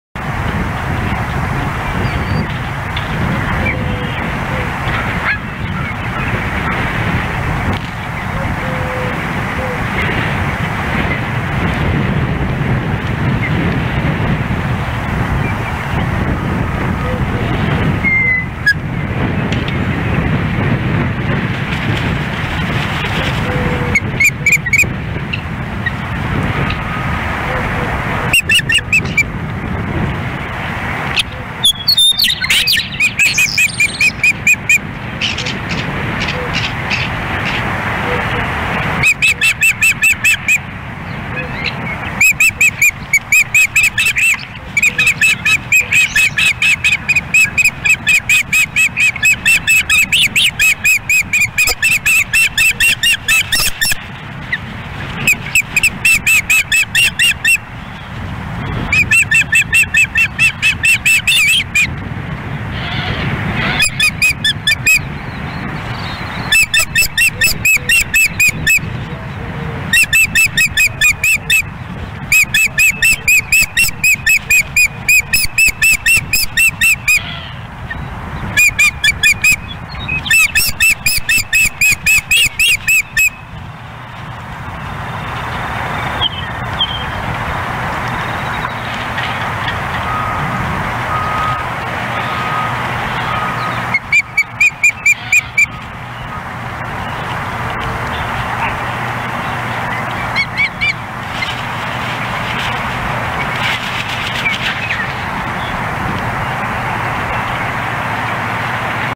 Dunrovin Ranch Ospreys ~ It's Not For Me, Hal? April 24, 2017
Courtesy of Days At Dunrovin, Dunrovin Ranch Ospreys in Lolo, MT at the nest of Harriet and Hal. April 24, 2017.  Hal brings a fish to Harriet but takes it away from her and leaves.